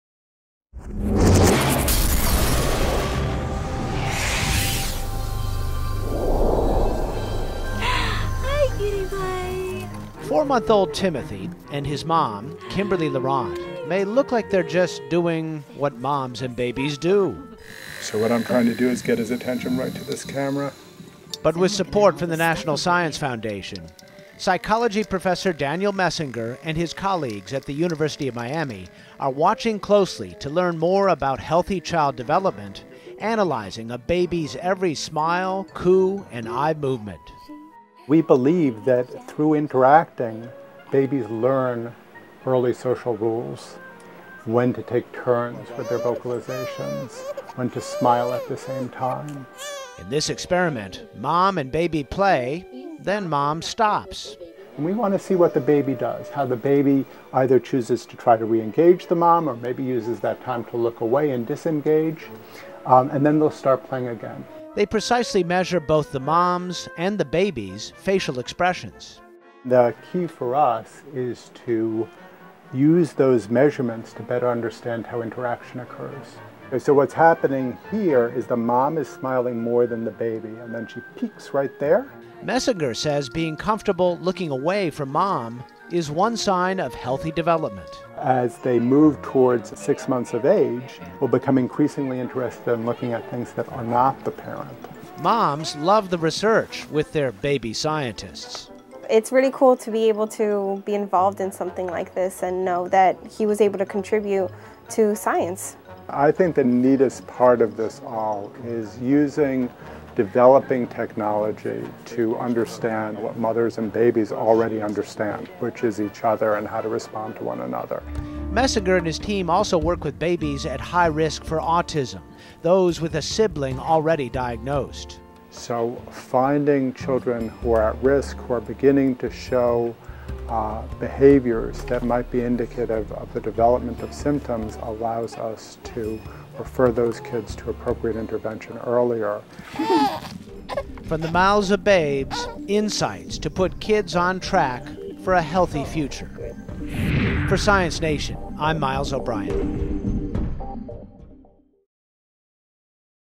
Baby Smiles Provide Clues to Healthy Development - Science Nation
Parents and babies smile, laugh and coo at each other, but scientists still have a lot of questions about how these interactions help infants develop.

"Parents tend to put a lot of emotional energy into these interactions," says University of Miami psychology professor Daniel Messinger. "And, the job of the baby is to do whatever they want, and they take that job very seriously."

With support from the National Science Foundation (NSF), Messinger and his colleagues want to learn more about child development by studying how moms, dads and babies respond to each other.

"We believe that through interacting, babies learn early social rules, such as when to take turns with their vocalizations, when to smile at the same time," says Messinger. "It's by smiling at the same time as their mothers, the baby responding to the mother and the mother responding to the baby, that babies develop a sense of shared social emotion."

"This is a very interdisciplinary team, including developmental scientists, machine vision experts, and face perception researchers," notes Peter Vishton, program director for Developmental and Learning Sciences in NSF's Directorate for Social, Behavioral and Economic Sciences. "The researchers are enhancing our understanding of social interaction during infancy and at the same time, developing tools that may transform how many other questions about early development are addressed."

In an experiment at the University of Miami's Early Play and Development Lab, babies are secured in a special seat, so they can get a good view of mom or dad, and move both their arms and legs. Babies are tested at four months, and again at one year.

Several video cameras capture this short, structured playtime.

"They play. Then, after two minutes, the mom will stop responding to the baby. We want to see what the baby does. How the baby either chooses to try to re-engage the mom, or maybe uses that time to look away and disengage, and then, start playing again," explains Messinger.

The videos are analyzed with a software program that precisely measures the facial movements of both the baby and the mom.

"So, we are actually automatically registering and tracking the movements of the babies' and moms' faces, as they interact in time," continues Messinger. "The baby is seeing the mom move her face and listening to what she is saying moment by moment, and this technology really lets us get at those moment-to-moment changes, and how mom is expressing smiles and joy, and engaging with the infant, and how the infant is responding to the mom."

The key, he adds, is to use those measurements to better understand how interaction occurs, and how babies learn early social rules.

"One of the things we found is that when a baby looks away from the parent, it just means they are interested in other things, it doesn't mean they are less interested in the parent. It just means they need to look around and see what else is going on," says Messinger.

Sairy Salazar, who is earning her degree in psychology, says she enjoys participating in the experiment with her daughter, Kelly. "I just like the exposure for others that can learn from it. If I can help somebody understand the mind, or the development of the children, I think that's very neat," says Salazar.

Along with studying healthy child development, Messinger and his colleagues also work with youngsters at high risk for developmental disorders.

The program Sibling Studies Measuring Infant Learning and Emotion, or UM SIBSMILE for short, investigates the social, emotional and cognitive development of children who are between the ages of two months and four years old.

"We have looked at babies who are at high risk for autism spectrum disorders in this lab, and these are kids who have an older brother or sister who has a diagnosed autism spectrum disorder," says Messinger.

"The younger siblings are at higher risk. Almost one in five of them will themselves develop an autism spectrum disorder, and that finding came out of the result of studies like our own and those of our collaborators and other investigators following up with these kids. We have increasing evidence that early intervention works with autism spectrum disorders," he says.

"Sensitive and responsive interaction with babies is what makes babies grow healthy," adds Messinger.

Miles O'Brien, Science Nation Correspondent
Marsha Walton, Science Nation Producer